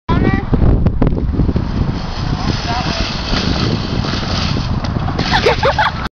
hit the lawn
rocke and connor running with a shopping cart trying to make it to the bushes